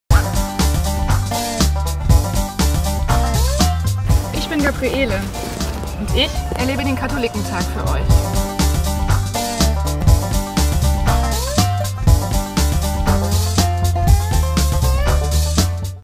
Gabriele Höfling: Für Euch dabei
Katholisch.de Reporterin Gabriele Höfling ist für Euch auf dem 99. Katholikentag in Regensburg. Sie saugt die Stimmung auf, trifft Gläubige, trifft Atheisten und wartet auf Eure Eindrücke. Ihr habt Fragen oder Anrgeungen? Dann schreibt Ihr auf Twitter (KT14) oder Facebook oder seid dabei.